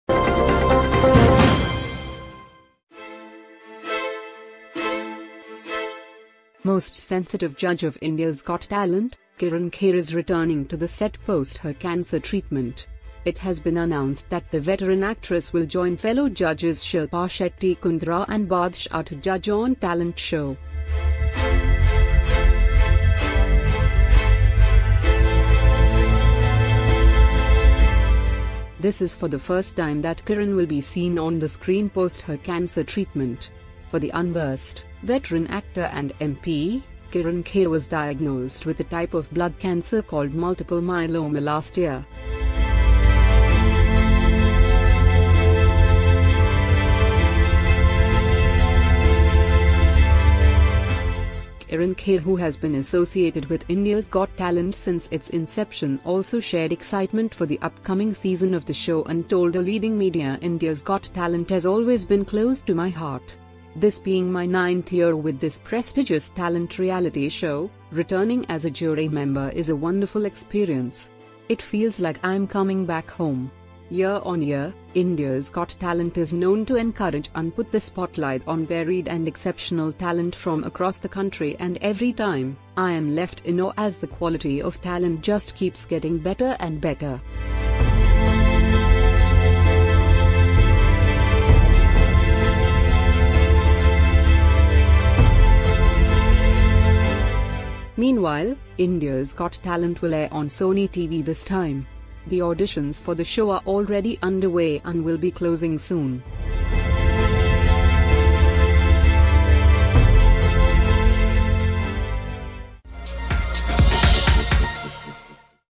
Kirron Kher returns to ‘India’s Got Talent’ post cancer treatment
The most sensitive judge of India’s Got Talent, Kirron Kher is returning to the set post her cancer treatment. It has been announced that the veteran actress will join fellow judges Shilpa Shetty Kundra and Badshah to judge on talent show.